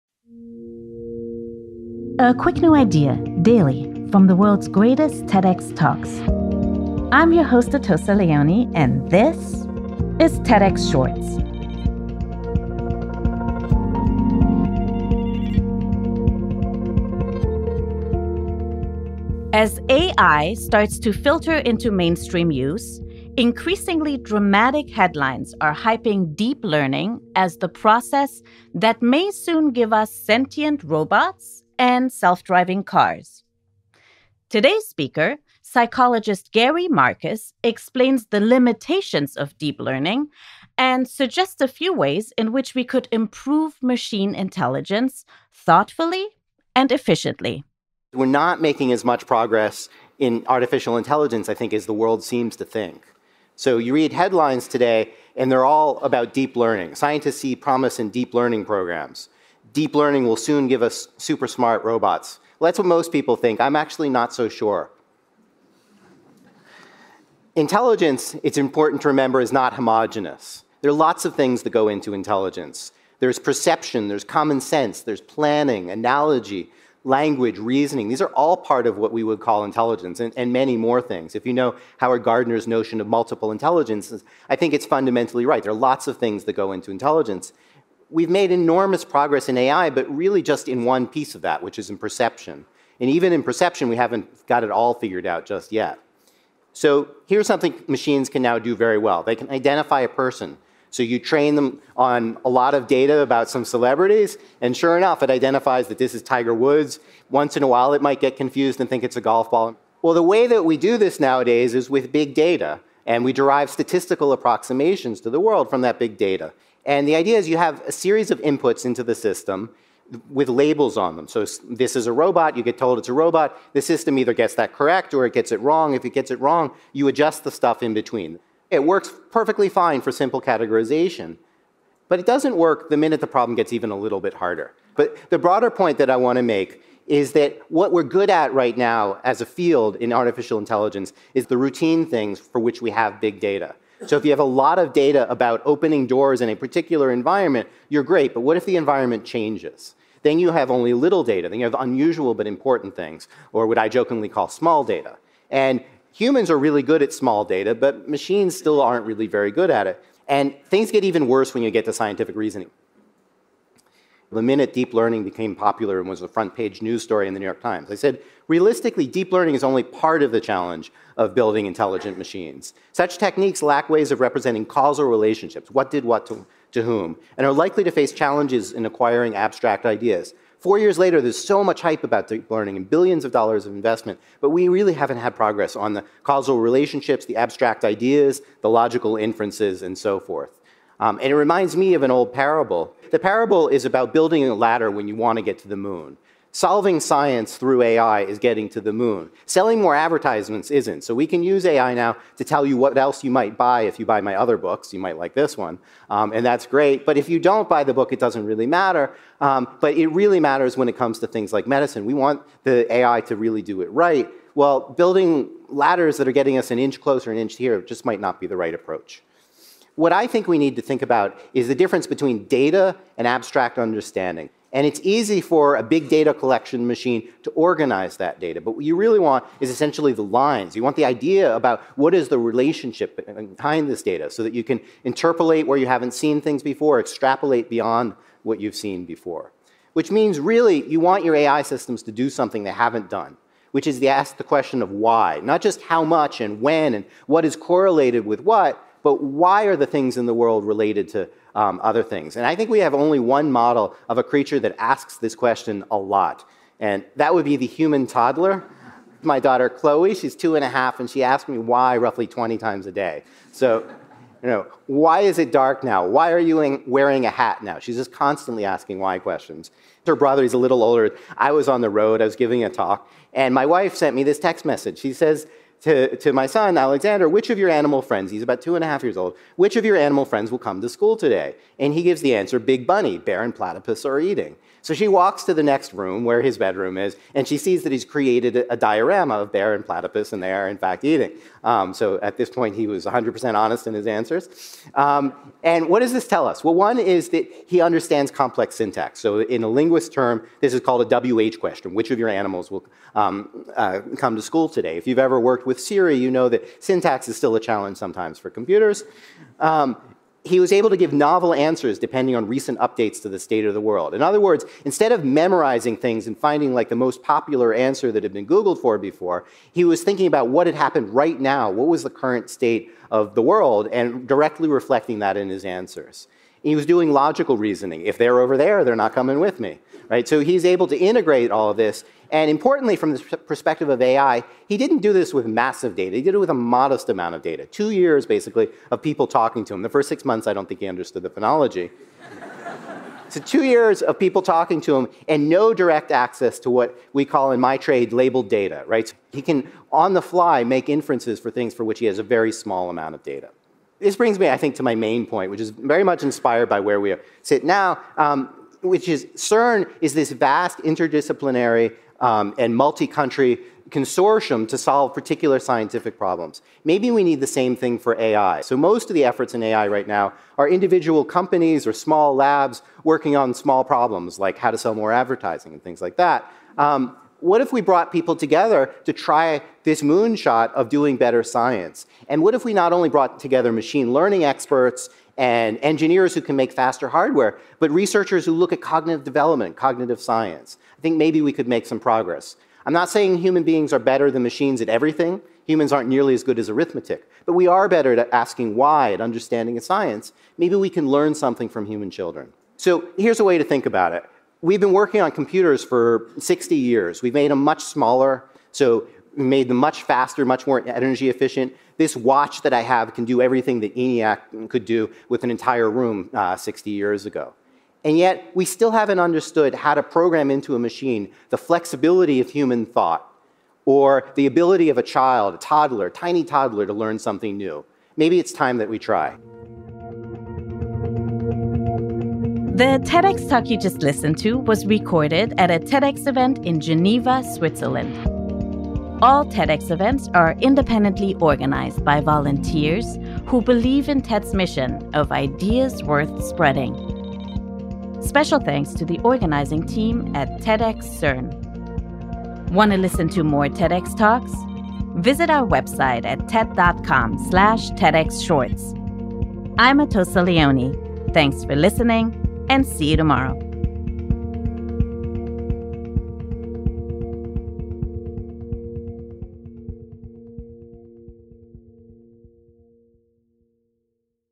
Why toddlers are smarter than computers | Gary Marcus | TEDxCERN
Gary Marcus dispels some current notions about AI advancements and suggests ways to improve machine learning. This talk was filmed at TEDxCERN.